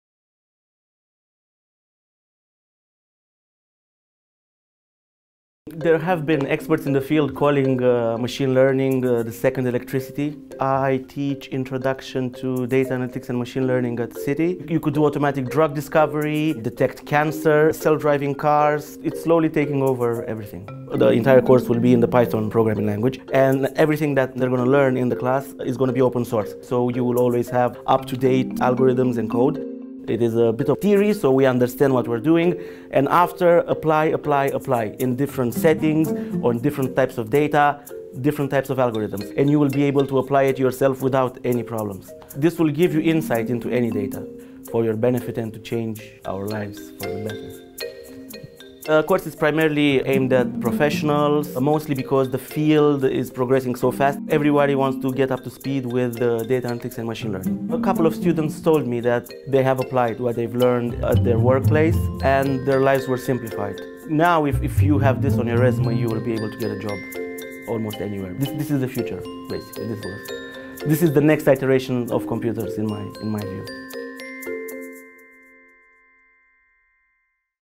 City Short Courses: Data Analytics and Machine Learning with Python
Is data analytics and machine learning the ‘second electricity’? City short course tutor and industry expert, Cosmin Stamate, explains why these skills are invaluable in the current job market.